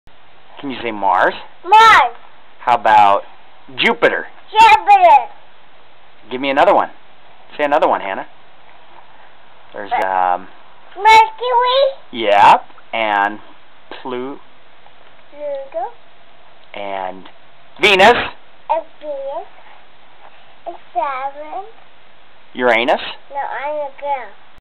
No, I'na Girl
little girl gives a new title to the seventh planet from the sun.